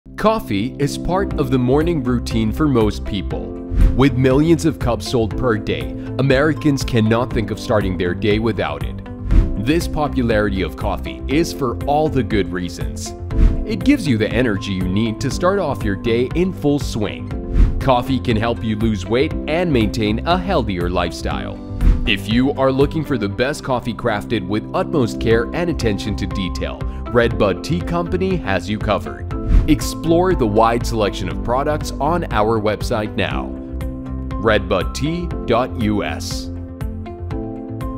Why your morning routine should include coffee
Starting your day off with coffee can make your day better by boosting productivity and giving to the energy you need. Learn more about the benefits of coffee in this video.